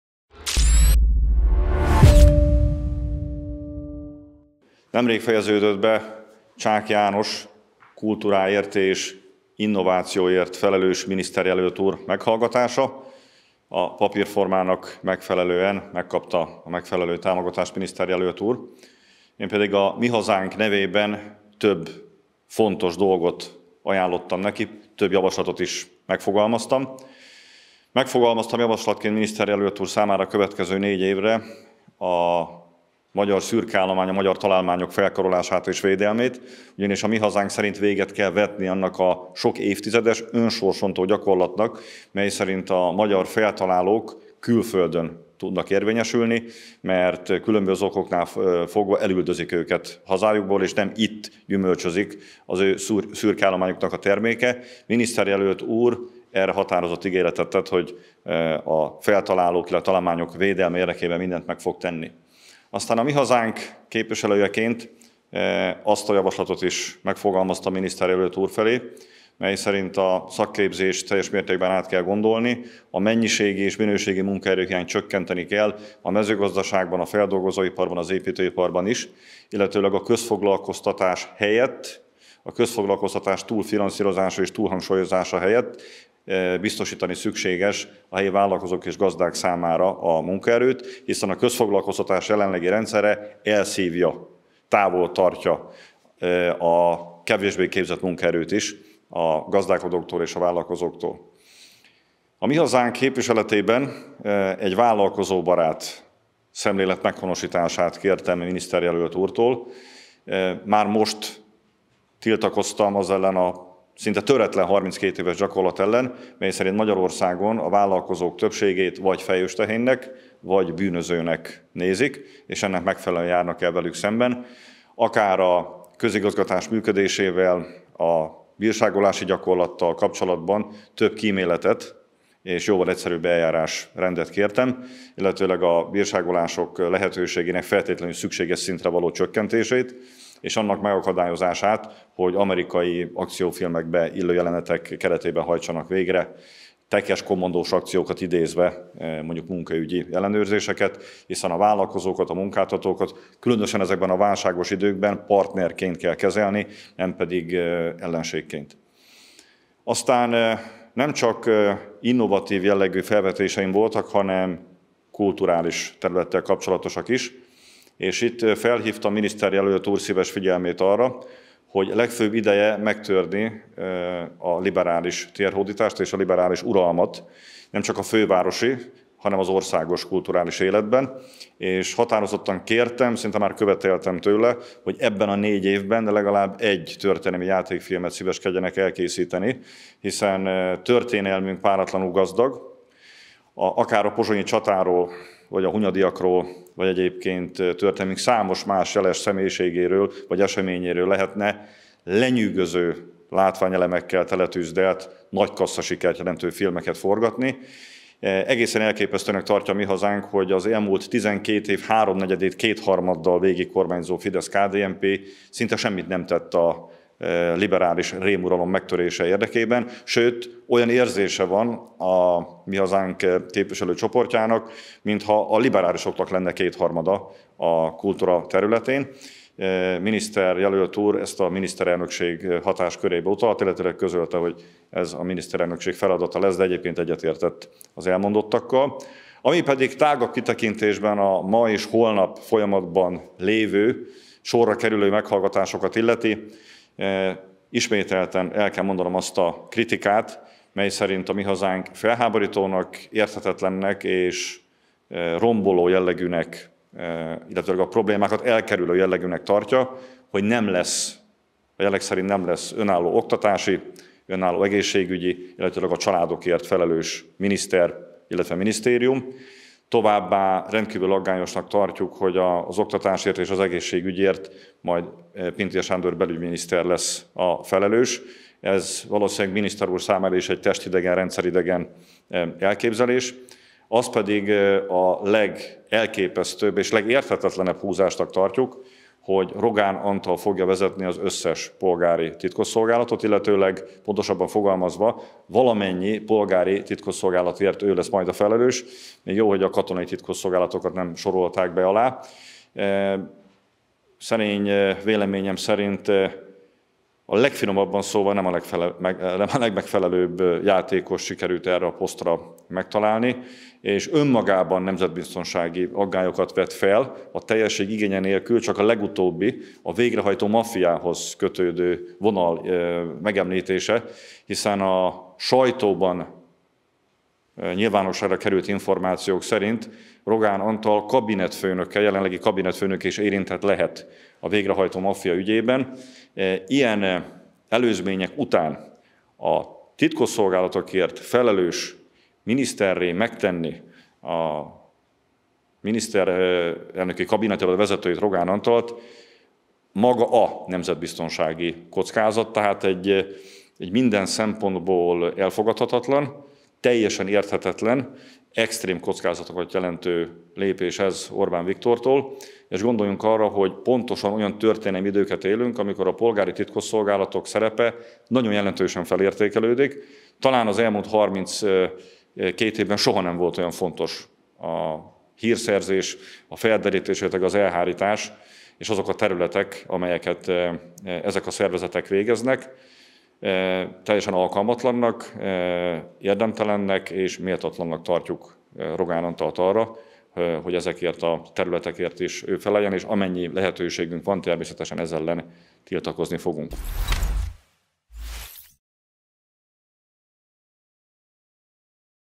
Reakció Csák János kultúráért és innovációért felelős miniszterjelöltre
A Mi Hazánk itthon tartaná a magyar feltalálókat, hiszen a jelenlegi önsorsrontó gyakorlat elüldözi a tehetséges magyarokat. Apáti István arra is felhívta a miniszter figyelmét, hogy itt az ideje megtörni azt a liberális hegemóniát, amely 30 éve uralkodik a kulturális életben. A Mi Hazánk nemzeti bűntudat helyett nemzeti öntudatot adna fiataljainknak, így szorgalmazzuk történelmi játékfilmek készítését. Alapanyagban nem lehet hiány, hiszen történelmünk páratlanul gazdag örökséggel bír. Apáti István megrótta a magát nemzetinek mondó kormányt, hiszen "a kultúra terén olyan, mintha még mindig a liberálisoknak lenne 2/3-uk" - fogalmazott a Mi Hazánk országgyűlési képviselője